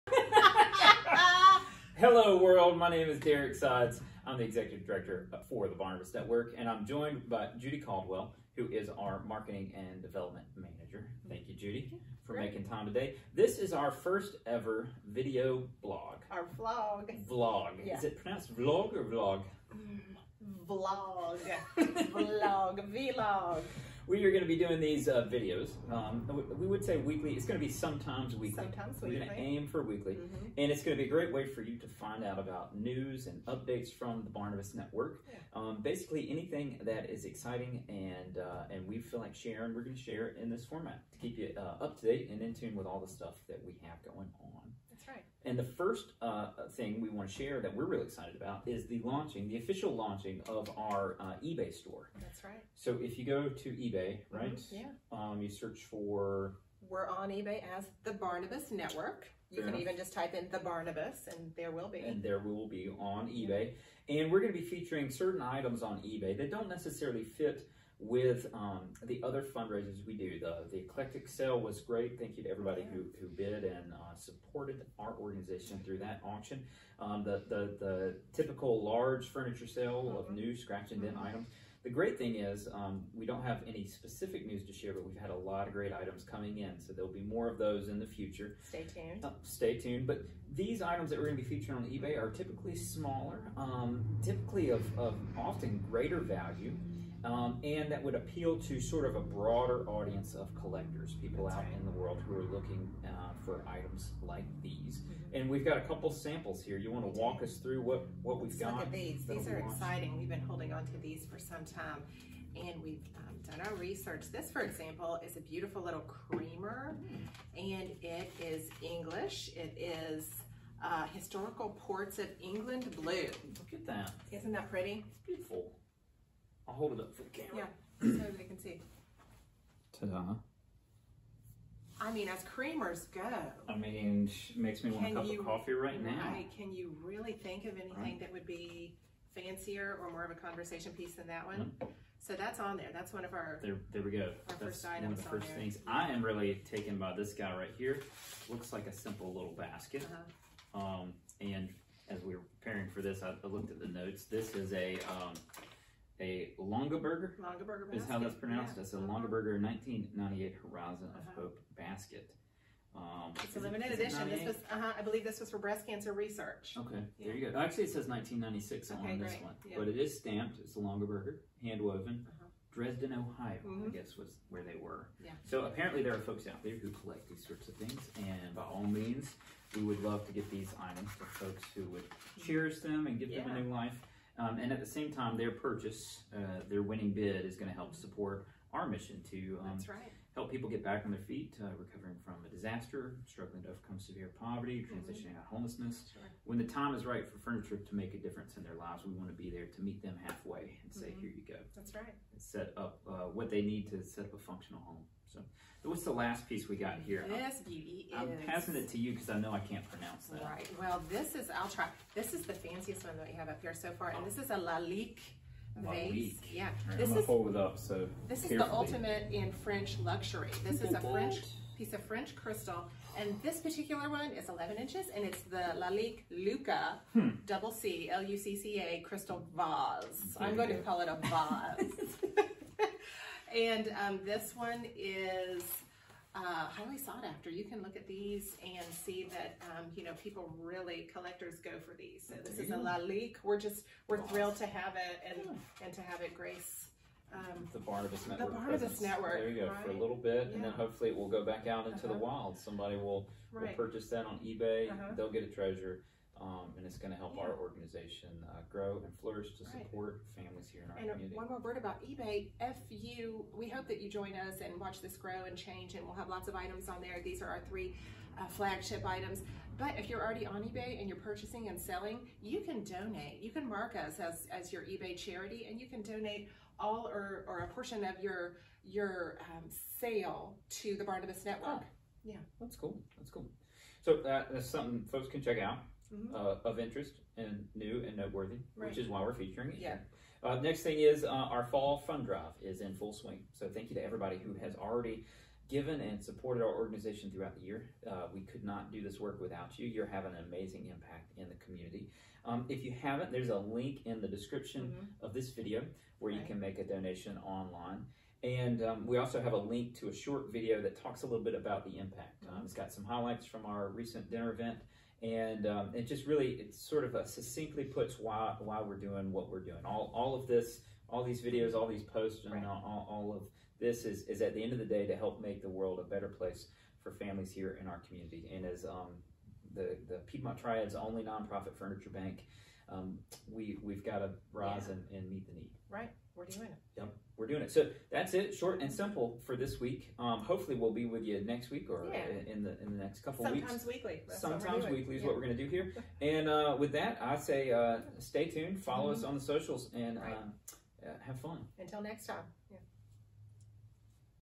Barnabas Vlog - Episode 1
Big news! Now is the perfect time. Join Derrick and Judy for the first-ever, sometimes weekly Barnabas Vlog! Or, if you want, call it a talk show.